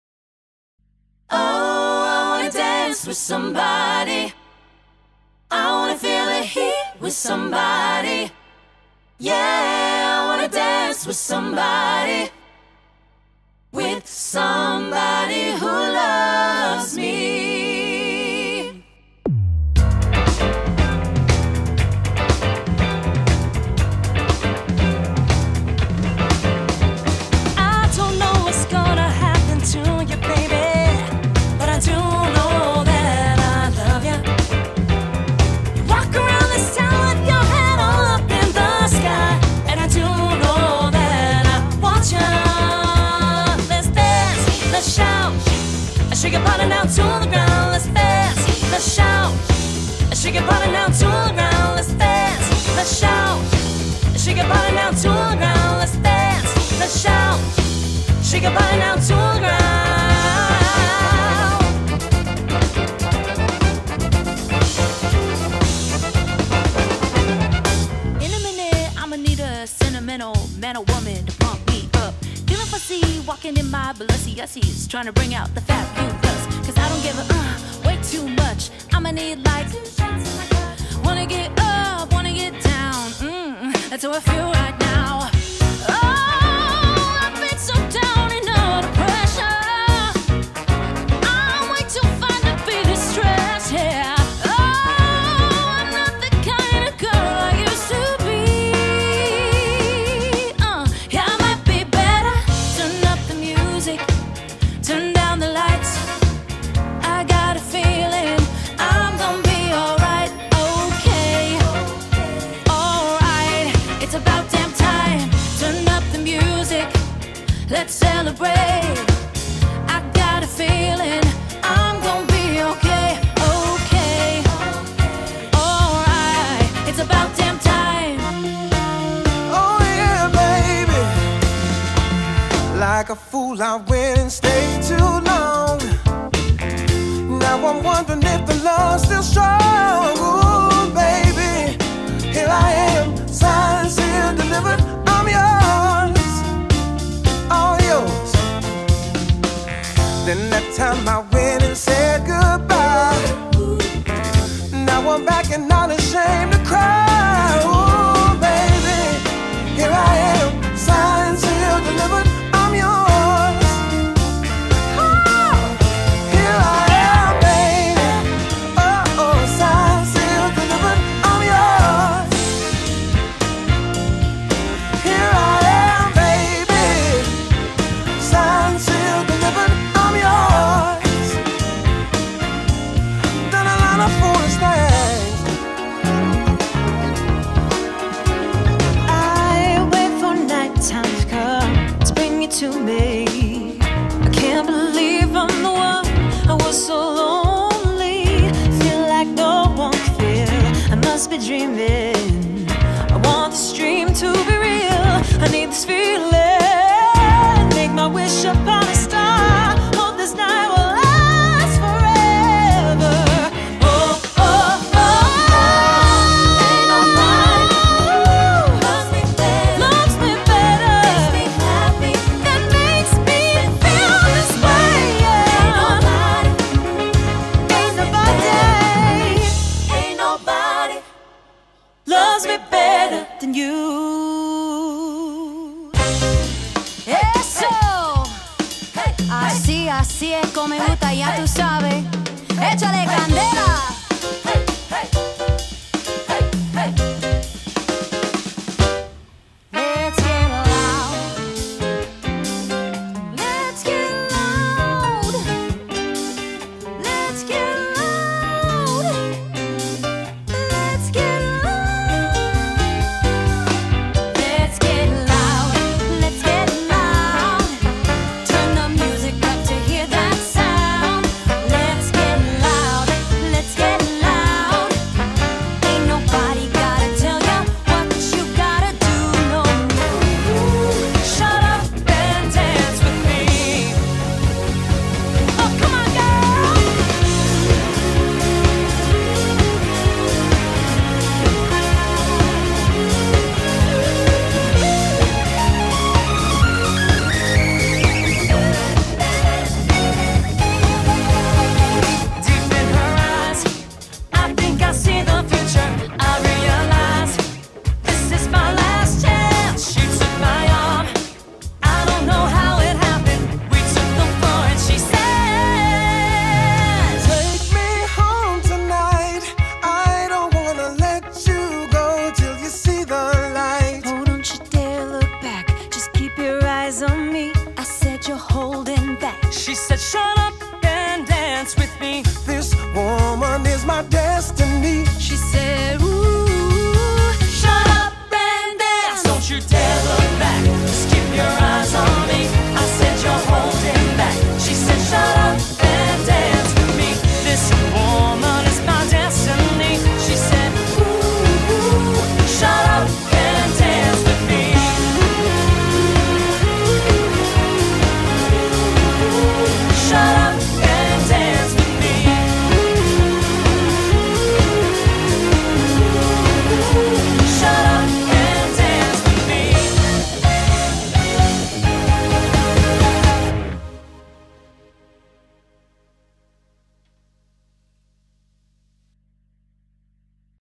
Vegas Cover Band- Live Music Society- Full Production Video
Live Music Society's full scale production band featuring all 5 vocalists in Las Vegas, NV.

This material is not our own, and is intended as cover music.